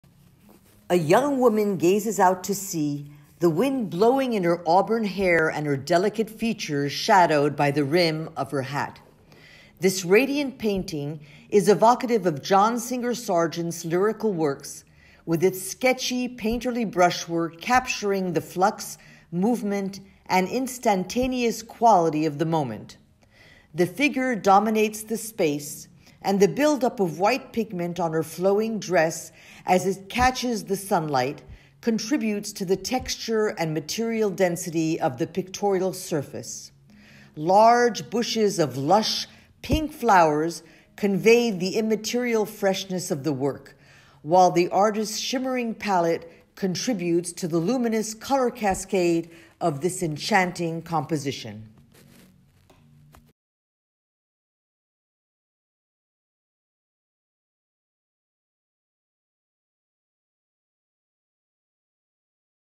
Jean Patricot (1865-1926): Young Woman Gazaing Out at Sea
Patricot’s loosely constructed compositions are lyrical and Impressionistic, infused with light that permeates the entire pictorial surface and dematerializes space and matter.